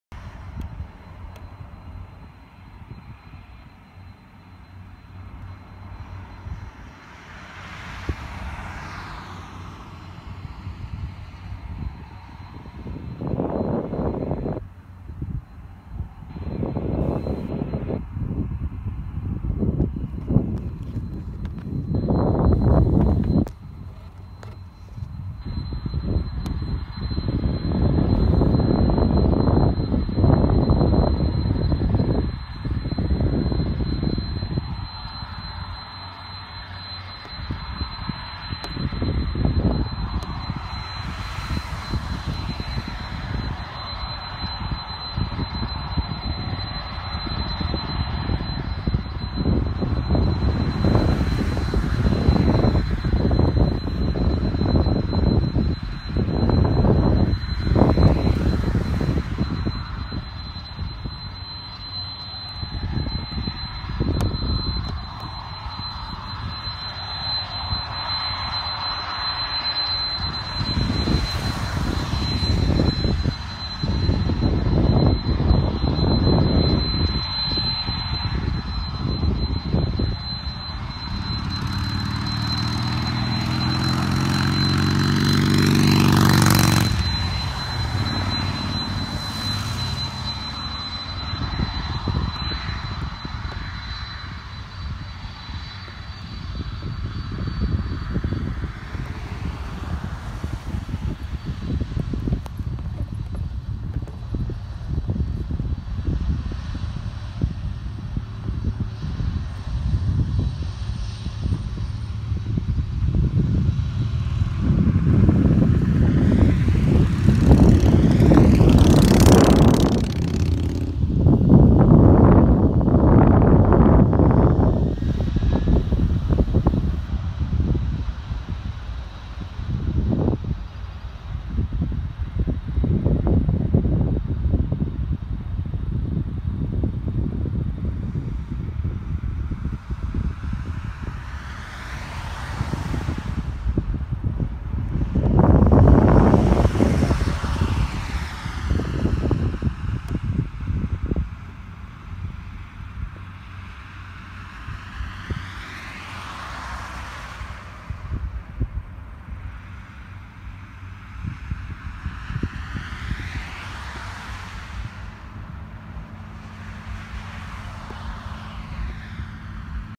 UPS 767-300 takeoff from Chicago Rockford International Airport on July 18, 2017
Mooney Takeoff from Chicago Rockford International Airport on July 18, 2017